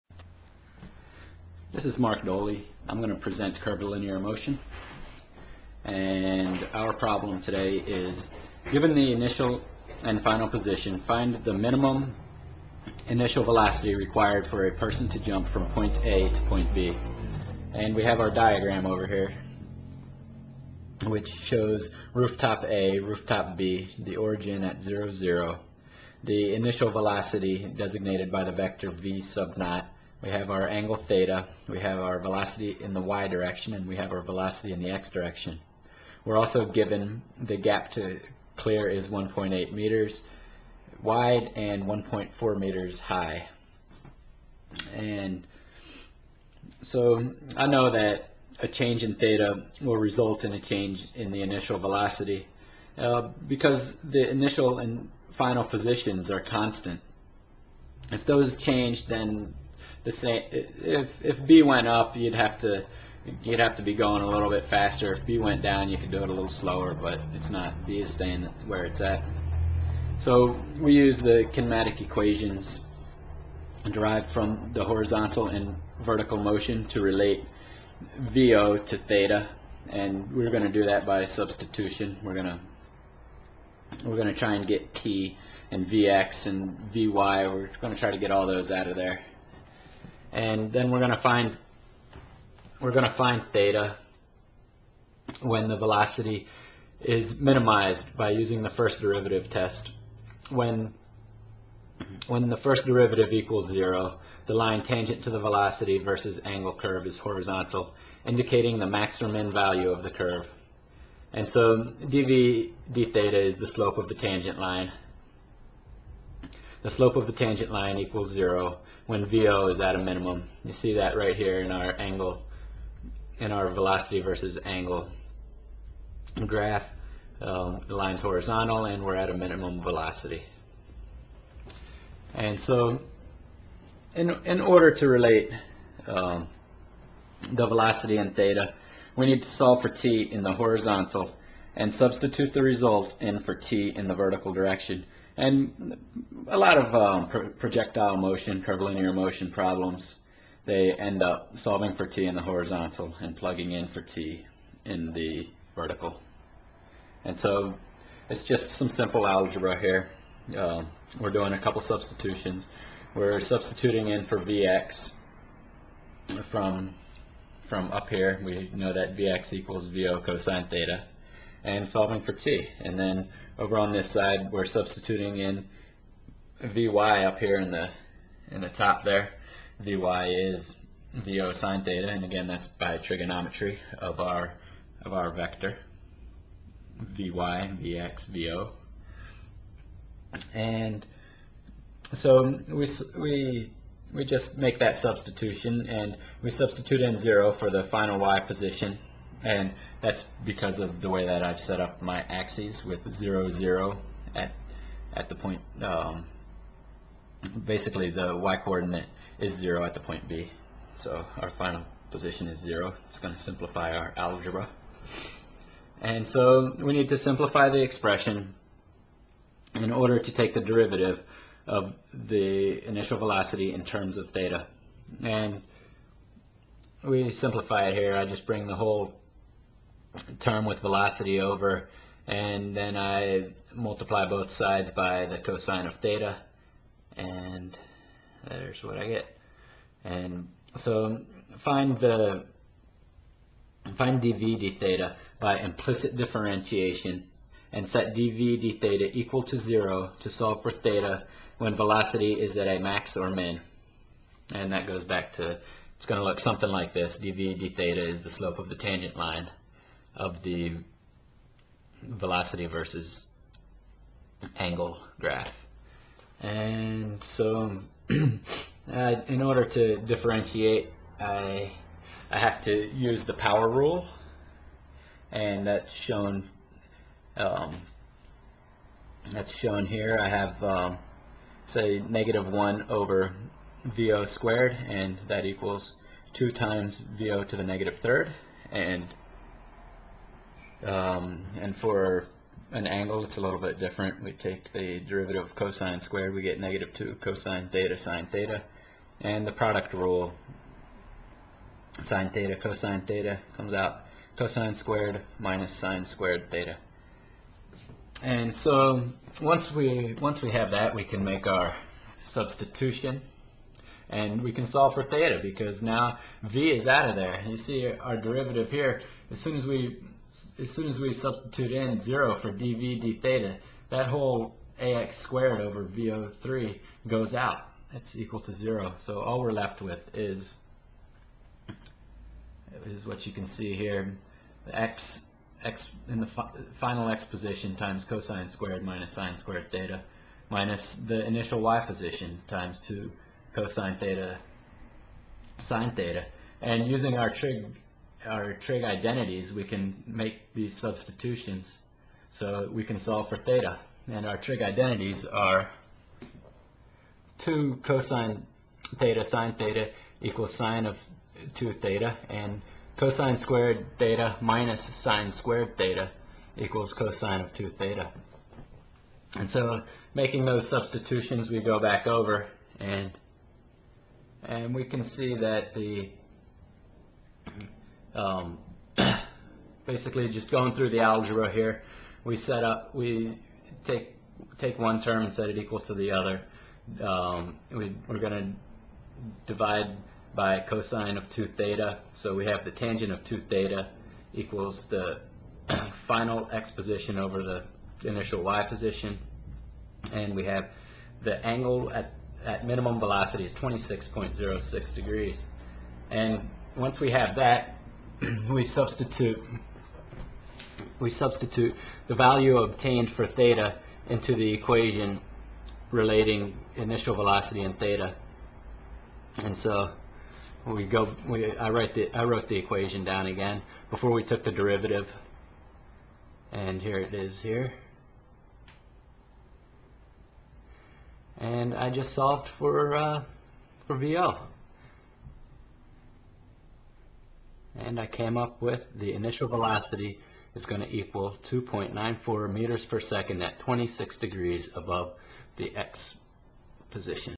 Curvilinear Motion
Find the minimum velocity required for a person to jump from roof "A" to roof "B".  Use the concept of curvilinear motion to develop a relationship between velocity and angle.  Then use the 1st derivative of velocity as a function of angle in order to minimize the velocity.